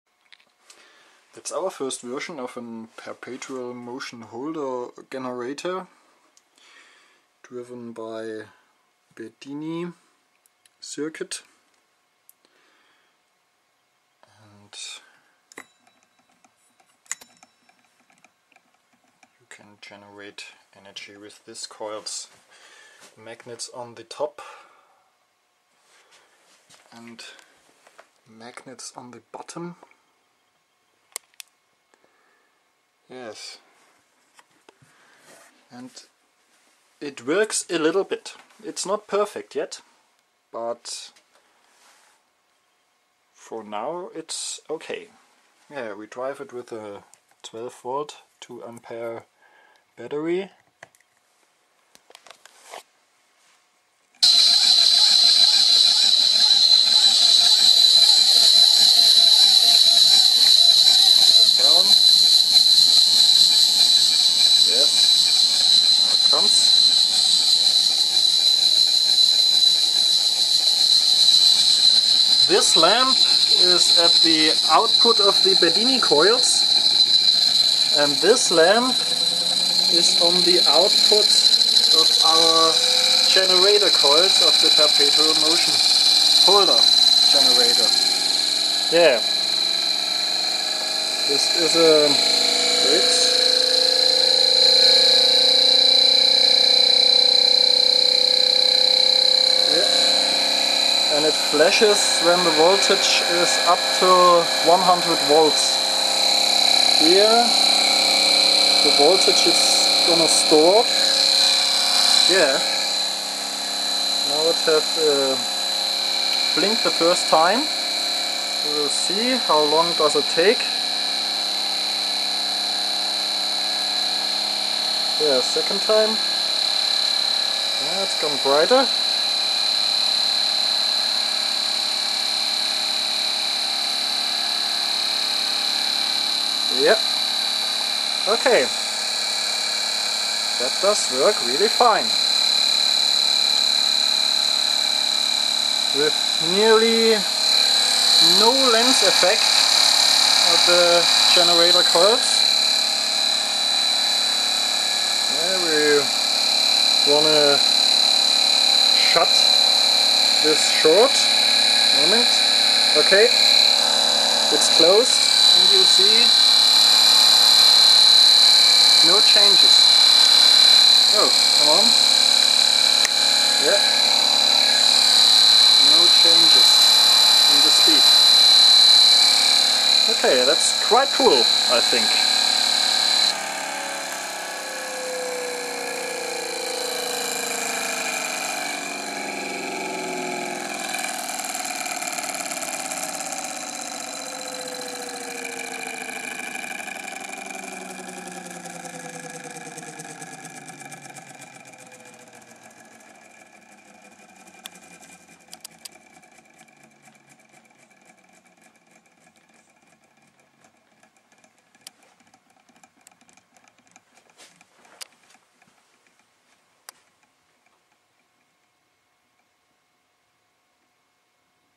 Perpetual motion holder generator
Our first version of a perpetual motion holder generator.
Search for Dr. Peter Lindemann and John Bedini to get details.